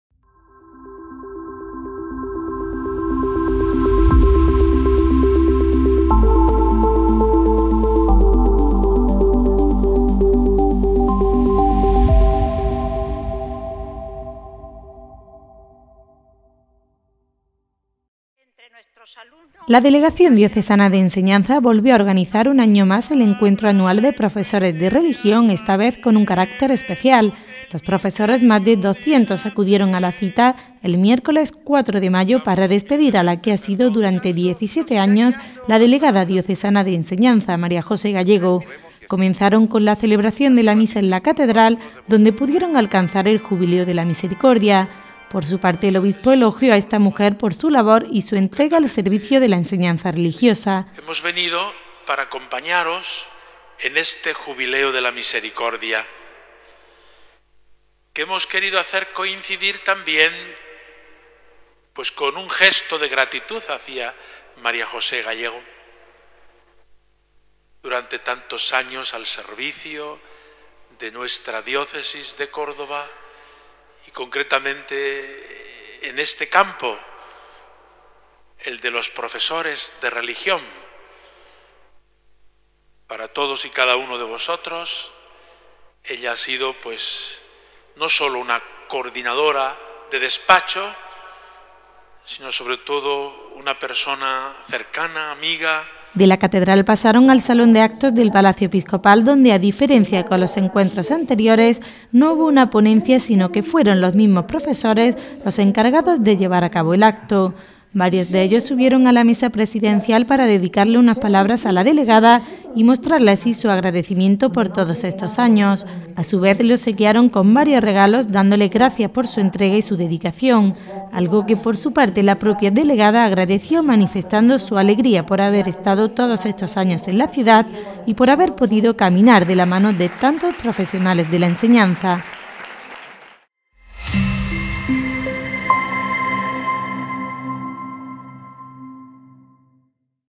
Los profesores de religión despiden a la Delegada de Enseñanza
La Delegación diocesana de Enseñanza volvió a organizar un año más el encuentro anual de profesores de religión, esta vez con un carácter especial. Los profesores, más de 200, acudieron a la cita para despedir a la que ha sido durante 17 años la Delegada diocesana de Enseñanza, Mª José Gallego.
Comenzaron con la celebración de la misa en la Catedral, donde pudieron alcanzar el jubileo de la misericordia y donde también el Obispo elogió a esta mujer por su labor y su entrega al servicio de la enseñanza religiosa.
De la Catedral pasaron al salón de actos del Palacio Episcopal, donde a diferencia con los encuentros anteriores, no hubo una ponencia sino que fueron los mismos profesores los encargados de llevar a cabo el acto. Varios de ellos, subieron a la mesa presidencial para dedicarle unas palabras a la Delegada y mostrarle su agradecimiento por todos estos años. A su vez, le obsequiaron con un cuadro, un álbum de fotos recordatorio y un ramo de flores, dándole gracias por su entrega y dedicación. Algo que por su parte, la propia Delegada, agradeció manifestando su alegría por haber estado todos estos años en la ciudad y por haber podido caminar de la mano de tantos profesionales de la enseñanza.